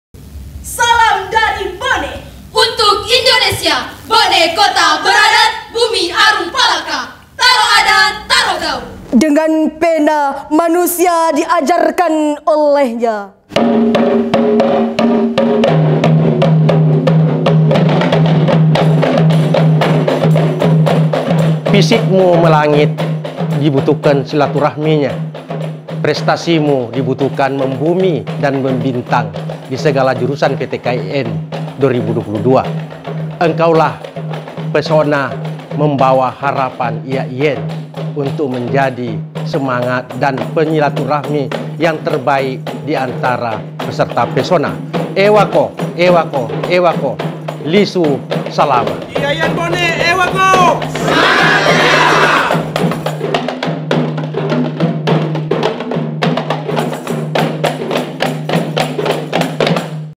IAIN Bone Goes to Pesona PTKN 2022
Gema Pesona semakin bergemuruh, setiap atlet yang akan berjuang mulai mempersiapkan diri demi hasil terbaik. Rektor IAIN Bone memberikan semangat dengan tebar pesona ke penjuru negeri.

Dukung Kontingen terbaik mu, dengan memberikan semangat api berkobar melalui kolom komentar.
Kontingen IAIN Bone , Siap menjadi juara?

"Spirit Of Harmony"